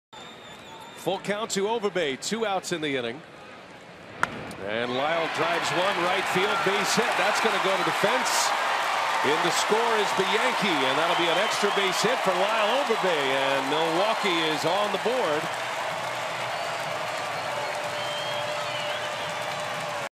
STL@MIL: Overbay delivers pinch-hit RBI double
7/12/14: Lyle Overbay delivers the Brewers' first run of the game on an RBI double, pinch-hitting for Tom Gorzelanny